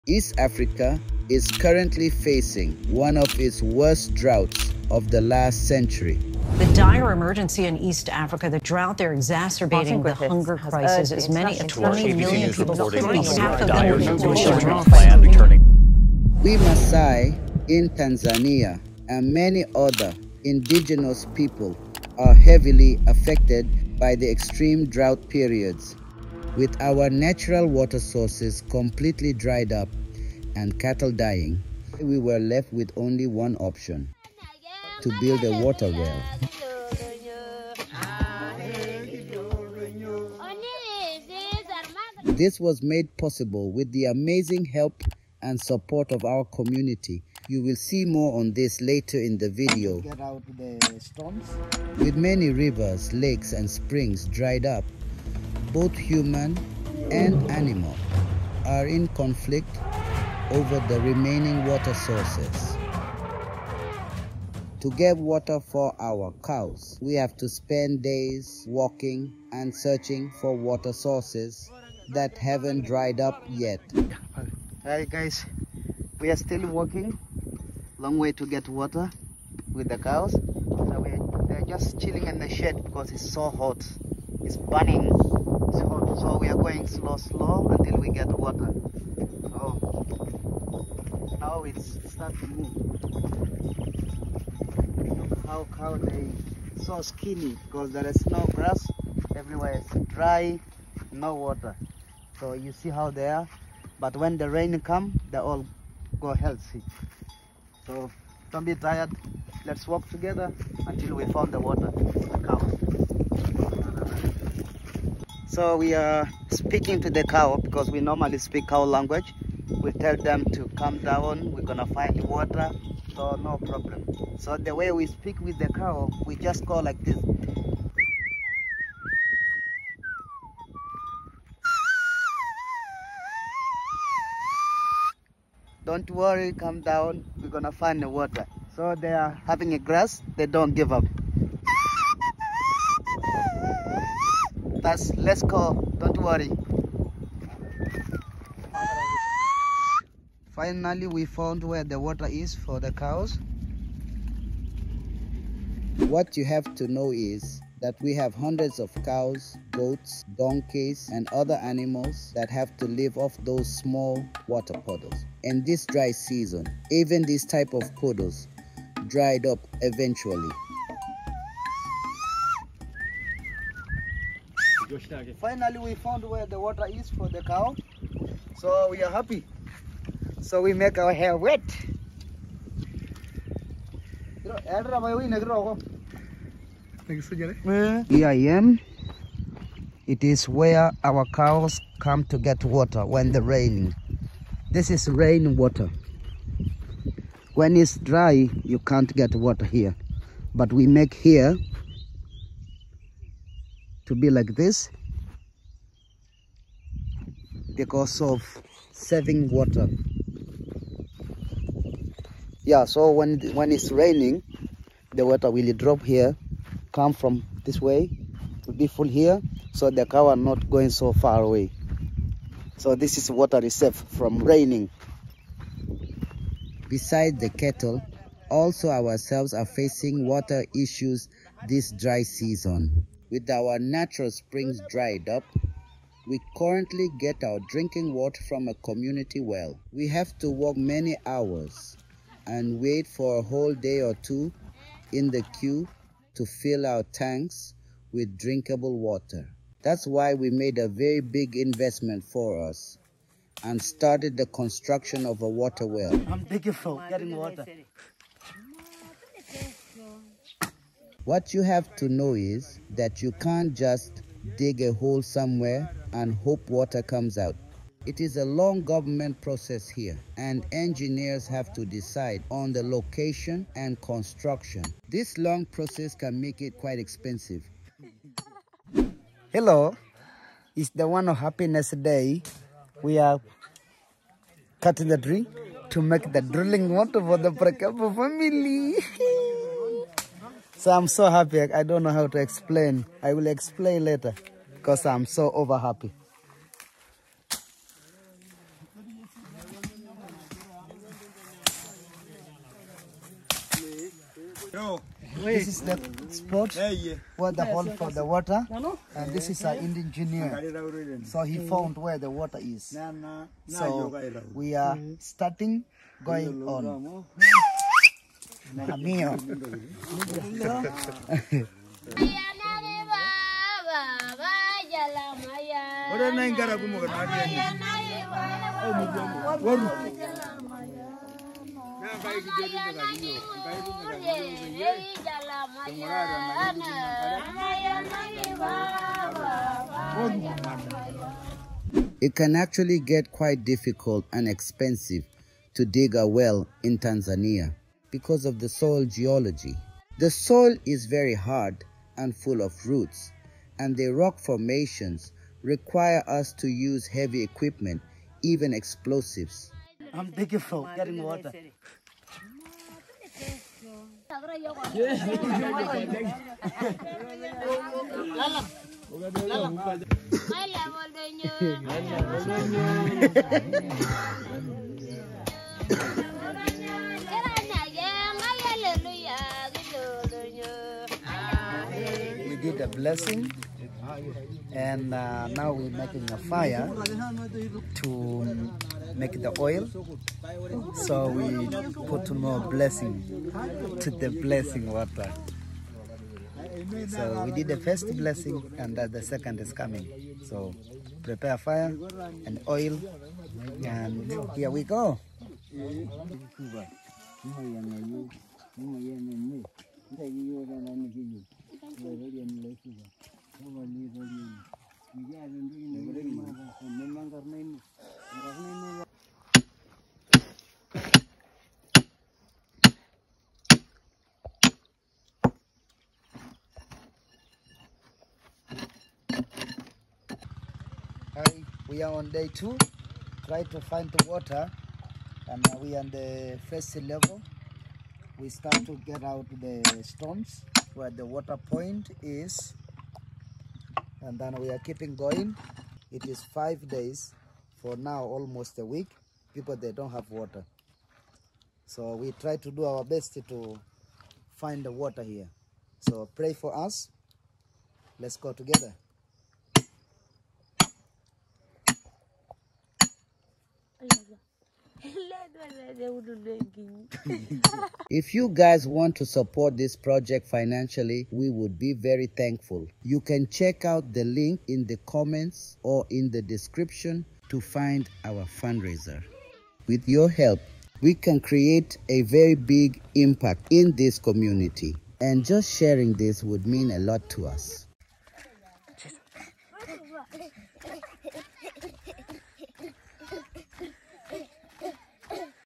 African Tribe Builds a Water Well
This Video is a little documentary on the drought situation in East Africa and how the indigenous Maasai peoples deal with situation.
East Africa is facing one of its worst droughts in the last century and we Maasai in Tanzania, and many other indigenous people, are heavily affected by it. The drought periods this year were extreme, with cattle dying and our water sources completely dried up. We have to walk for hours and stay for days sometimes to fill our tanks with drinkable water.
That is why we were left with only one option -to build a water well.
In this video we will give you a good impression on how such a construction is done in Africa and a unique insight into the first hand experience, of people affected by climate change and droughts.

 What you need to know is, for us Maasai, Nature and our animals are everything for us and when this is at the brink on extinction, we get no sleep. We heavily depend on water for our cattle and ourselves and the situation is threatening our traditional lifestyle and Culture.

We finished the first steps of construction now and if you guys are interested in supporting this project we would appreciate it a lot! We are very thankful for every donation and just sharing it means the world for us.